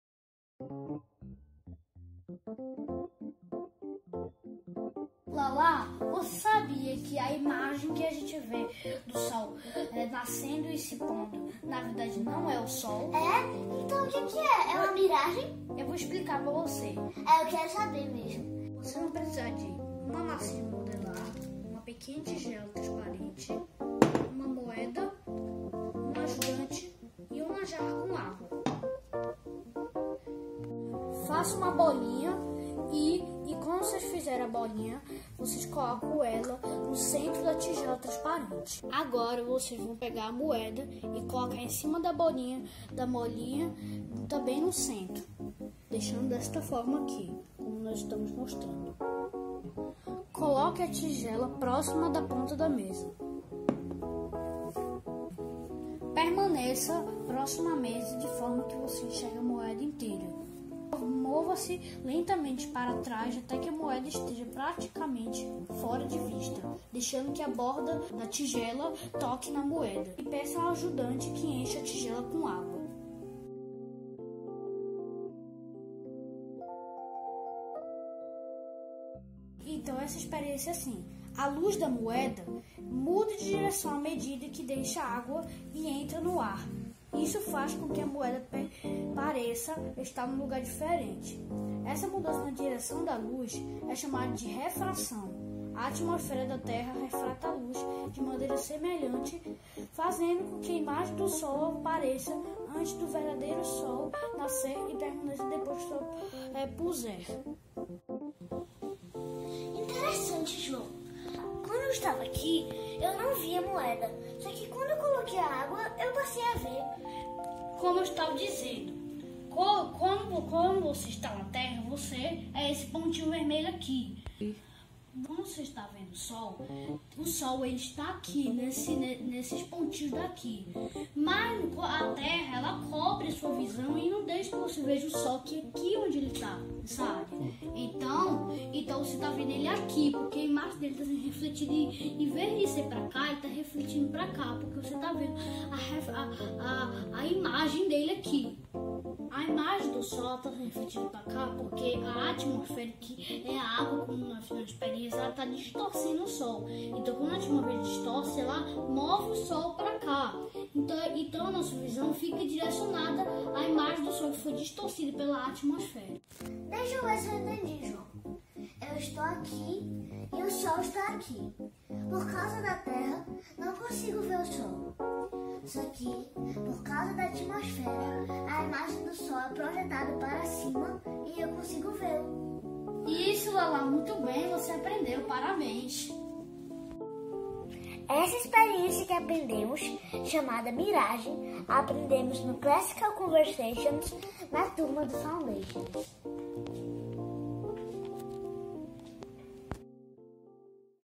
Experimento de Ciências - Refração
No Classical Conversations as crianças do Foundations aprendem ciências na prática, observando e fazendo experimentos.

O Foundations é o nível do conhecimento e, na ciência, é o nível de observação. Nós fornecemos à criança o tempo, as ferramentas e as oportunidades de observar e explorar. 'Os estudos da ciência podem ser orientados pelas preferências das crianças, e complementados com experiências, coleções, estudos da natureza, textos copiados, arte e leitura" (Bluedorn).

No Foundations, estimulamos as famílias a fazerem experimentos simples em casa. Isso ajudará a desenvolver ideias para projetos científicos quando a criança estiver mais velha.

Assista ao vídeo e pratique em casa com seus filhos. Depois, assista na Internet a uma boa aula sobre o tema e aproveite para falar da grandeza deste Criador e de como Ele nos presenteou colocando em nós a Sua imagem e semelhança!